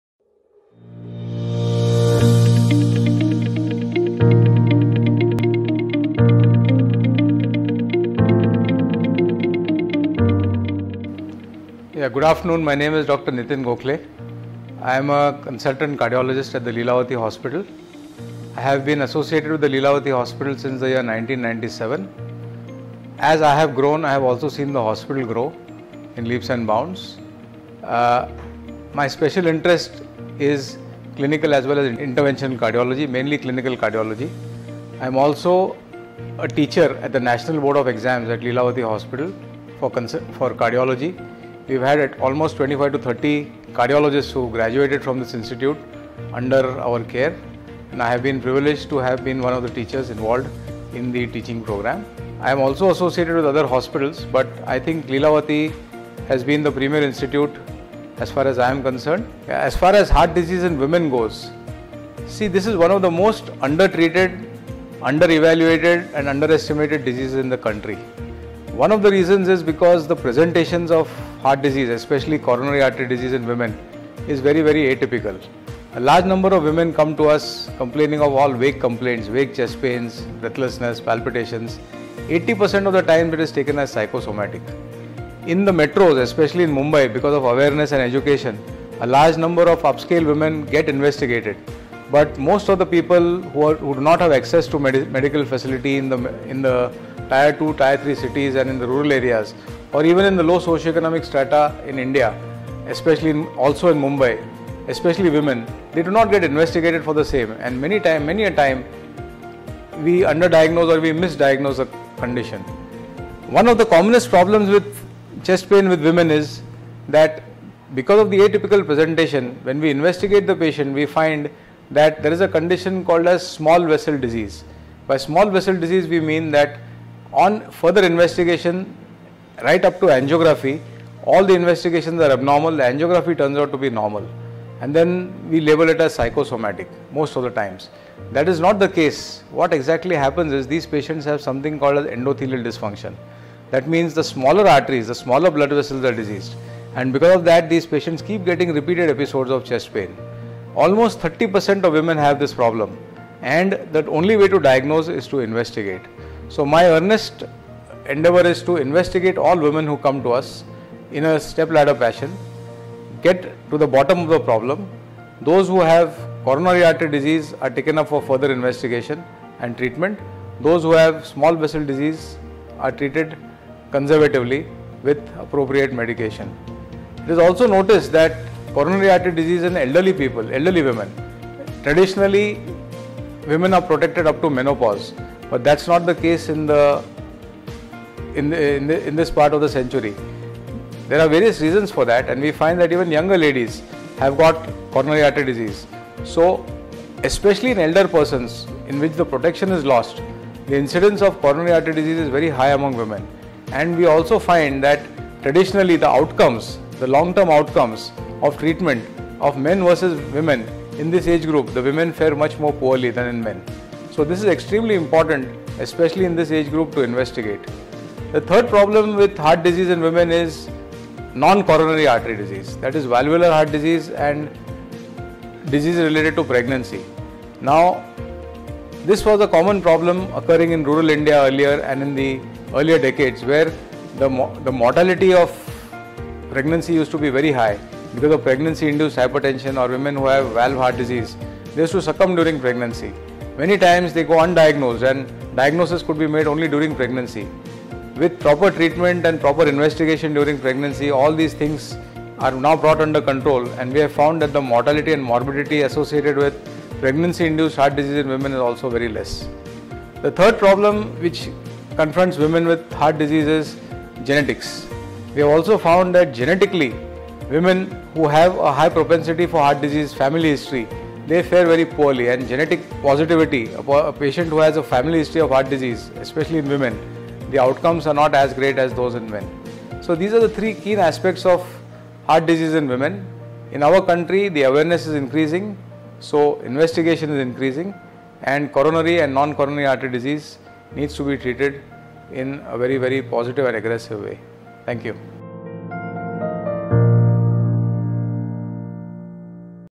Dr. Nitin Gokhale, Cardiac Surgeon
Watch our cardiac expert, Dr. Nitin Gokhale, a Cardiologist speak about major symptoms of heart diseases occurring in women.